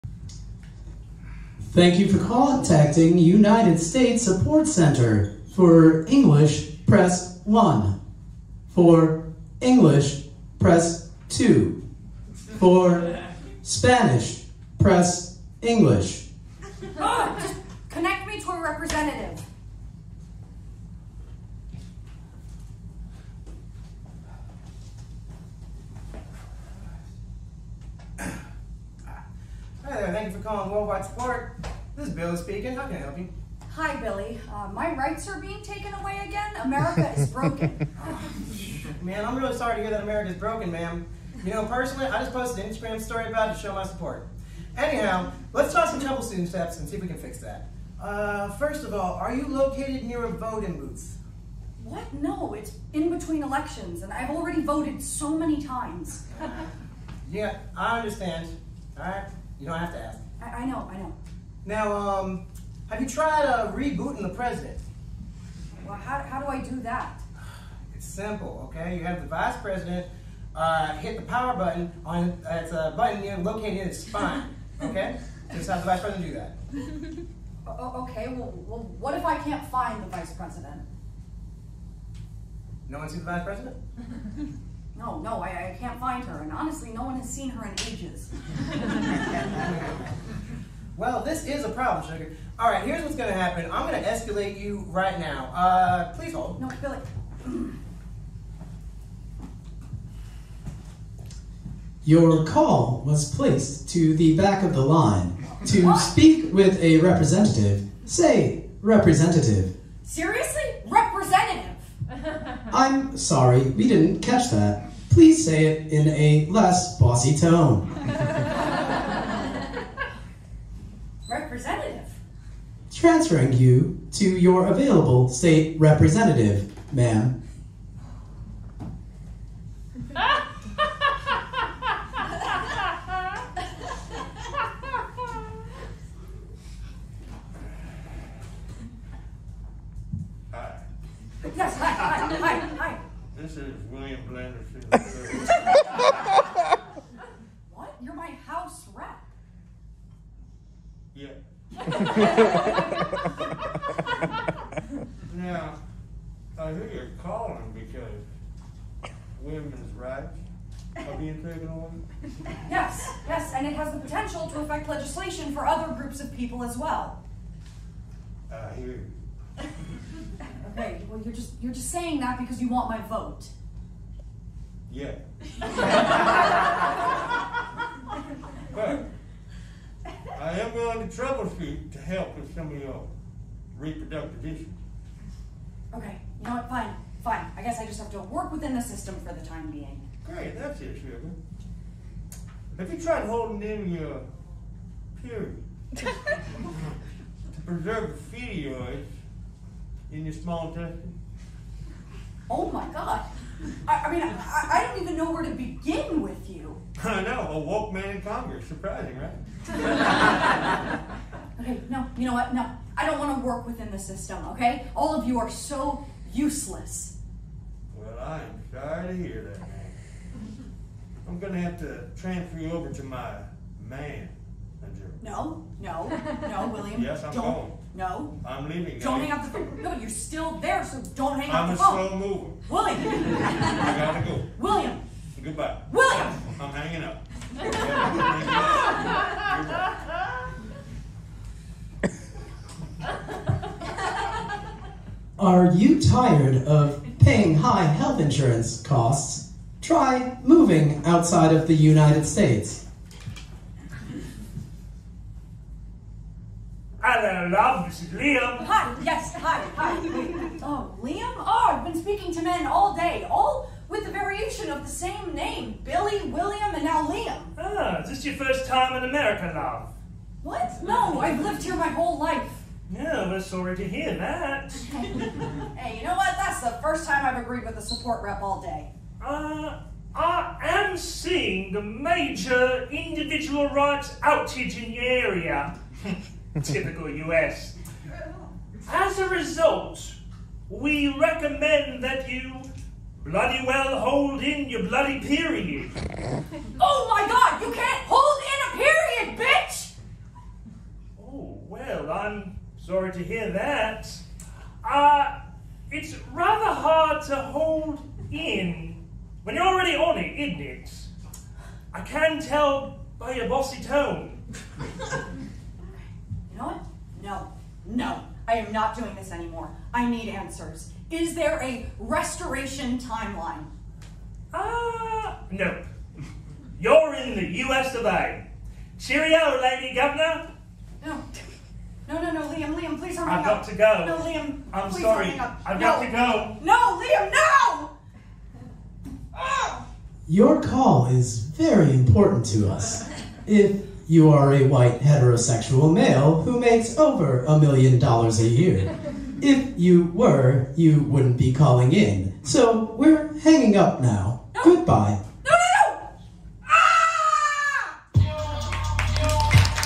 US Support Line - Cliff Hanger/Season 9/Show #3 (Sketch 2/7)
Written by Lisa Winters

Sarah - Alex O'Shea
Support Line Robo Speaker - Peter Goitom
Billy - Eddie Dougrou
William - Lane Koivu
Liam - Michael Newman

Directed by Steph Leschek